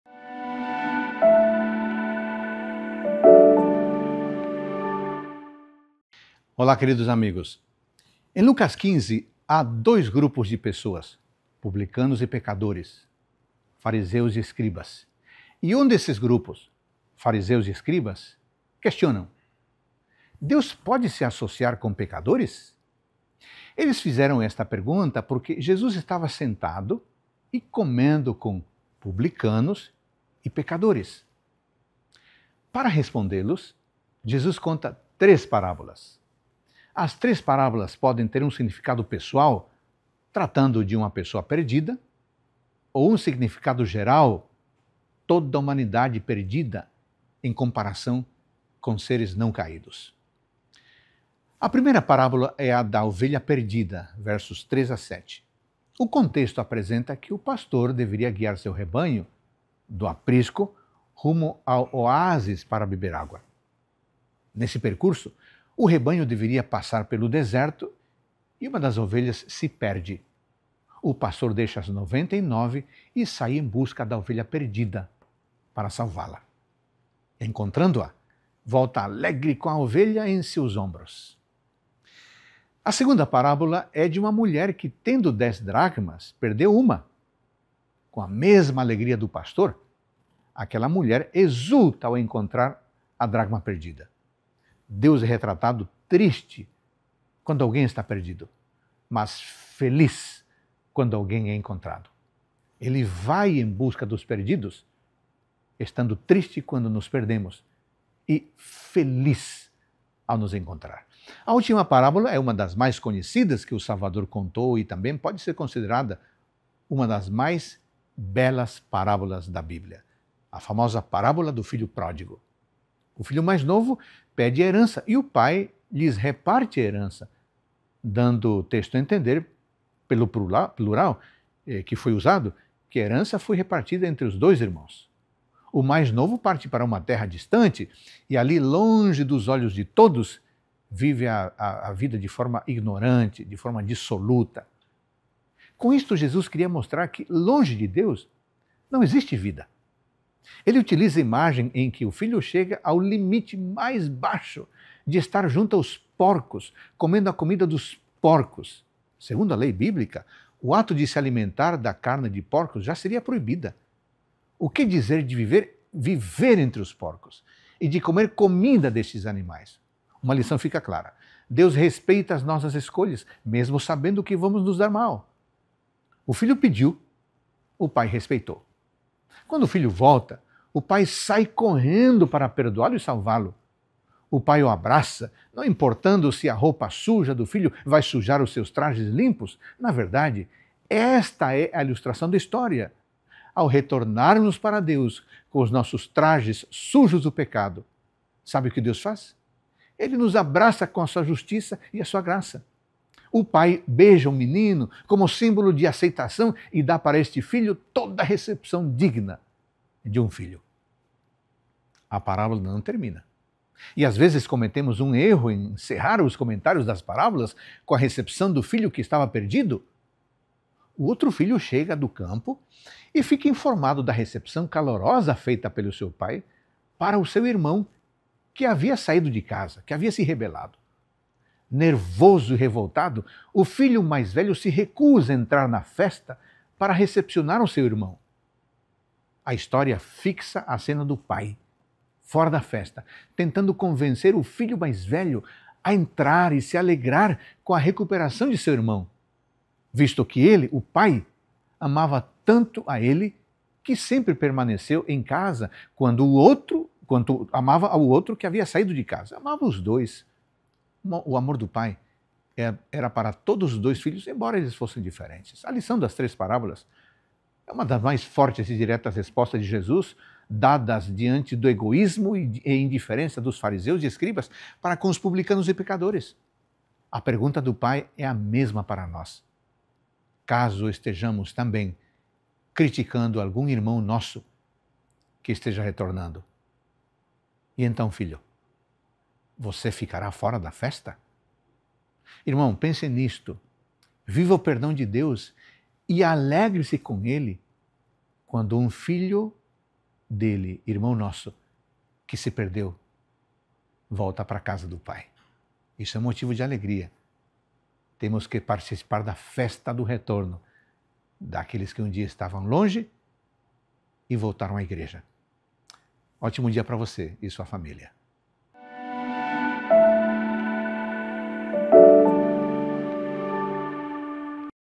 Lucas 15 - Reavivados Por Sua Palavra
A lição das três parábolas foi uma das respostas mais fortes e diretas que Jesus deu ao egoísmo e à indiferença dos fariseus e dos escribas para com os publicanos e pecadores.